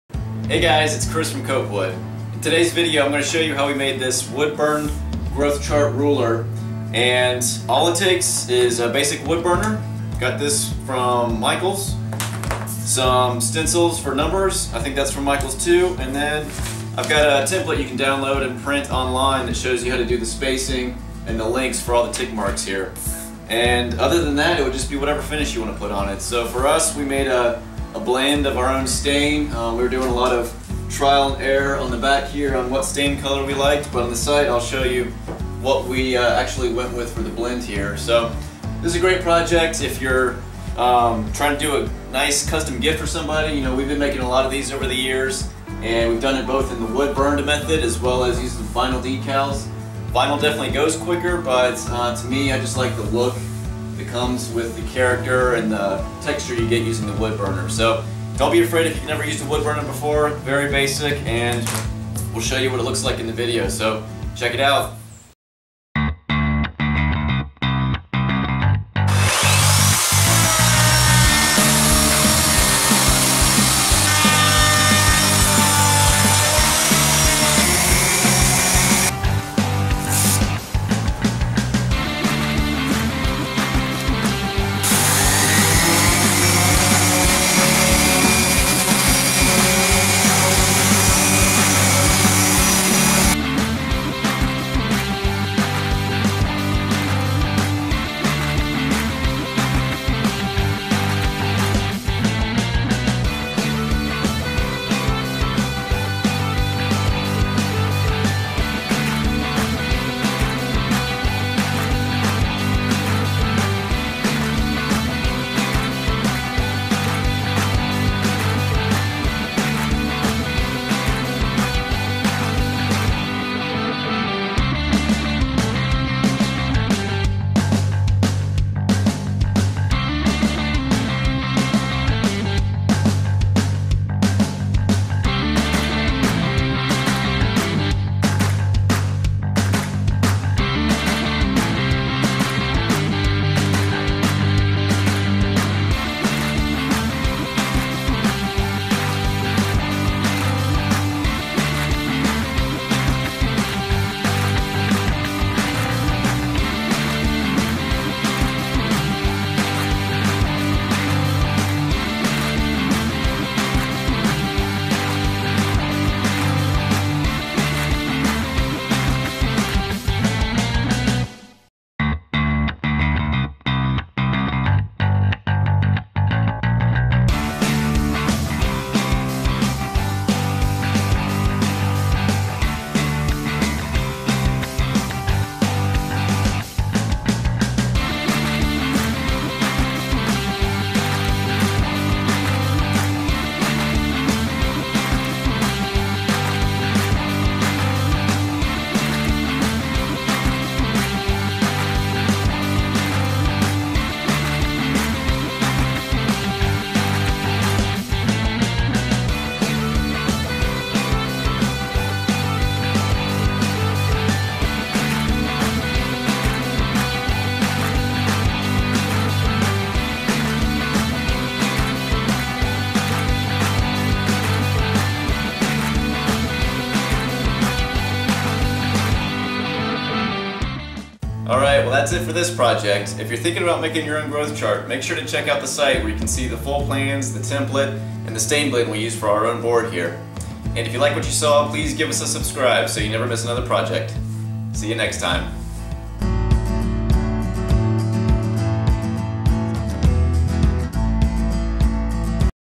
How to Make a Wood Burned Growth Chart Ruler | Copewood
Step-by-step video and FREE printable template for making a DIY Wood Burned Growth Chart Ruler. No previous experience with wood burning required!